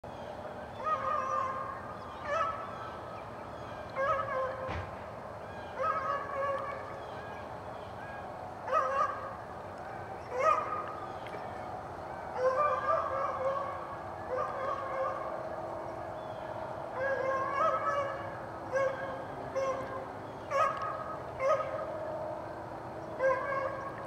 Dogs Fence Running
Two dogs fence running, fence chasing with a third dog on other side of fence.

Fence running is a common dog activity that is often misunderstood by owners. Contrary to popular belief, it is not a good method of exercising a dog. Even though it looks like the dogs are enjoying the chase, the frustration caused by the fence often results in dog aggression.